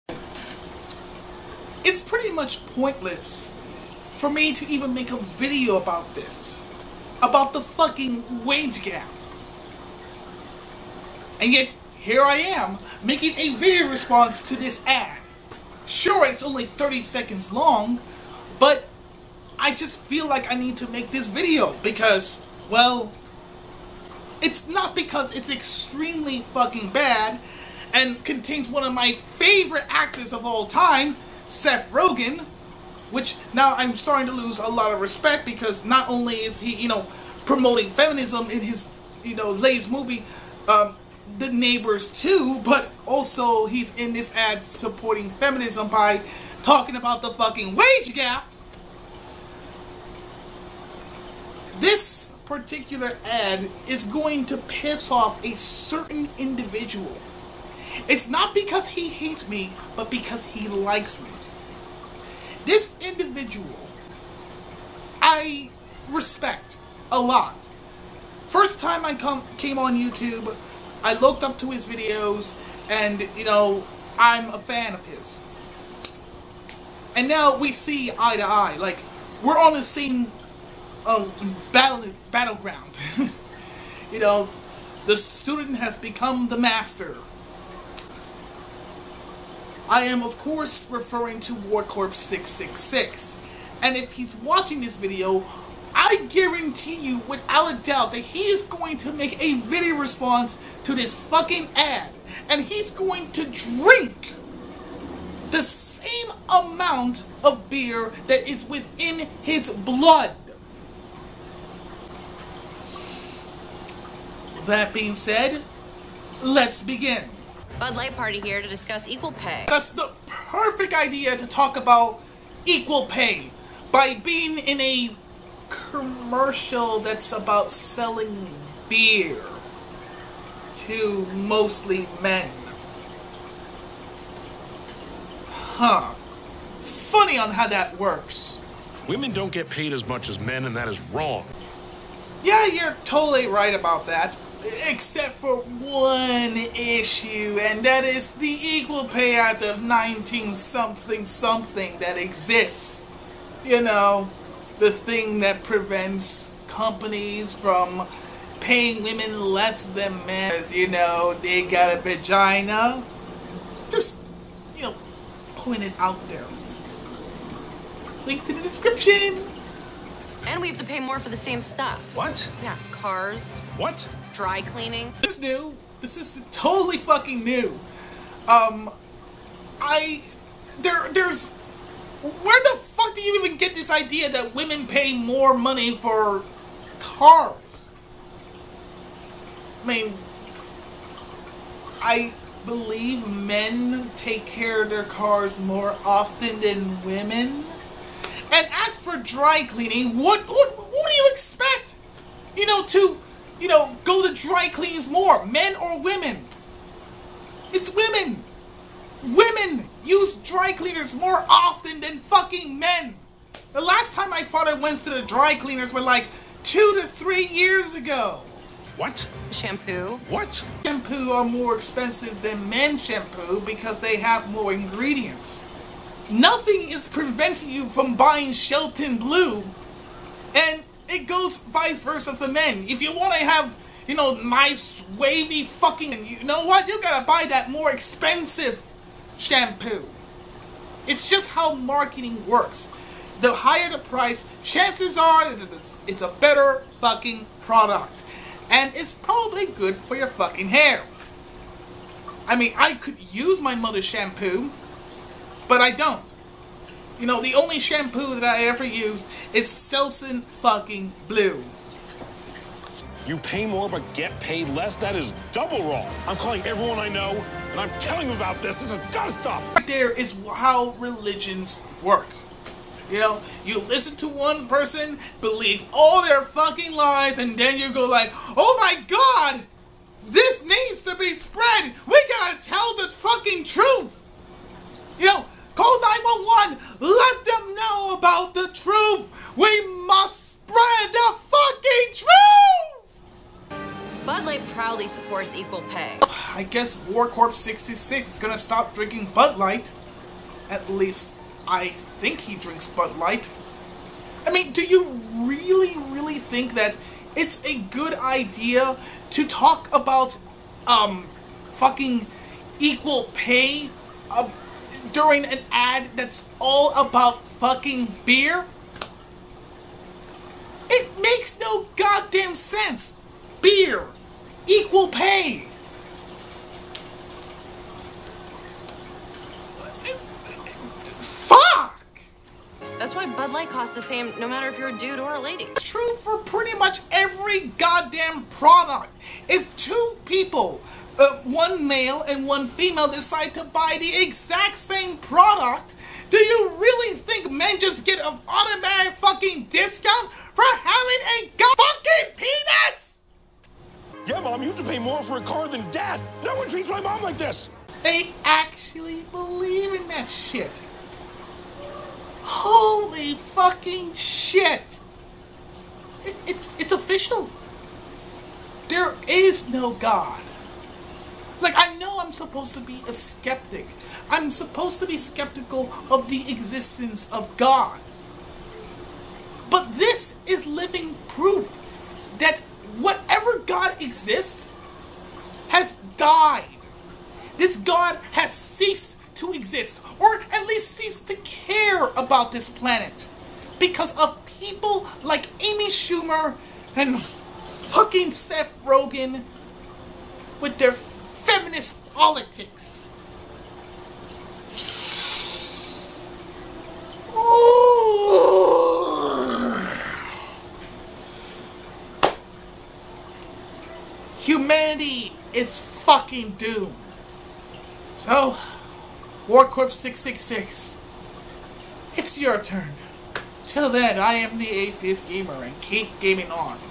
Bud Light Political Ad For "Equal Pay"
I am not making this video because it an extremely dumb ad (not to mention it makes no sense) but because i know warcoprse666 is gonna be pissed off to find out that this add exist and he will end up drinking beer in his own weight.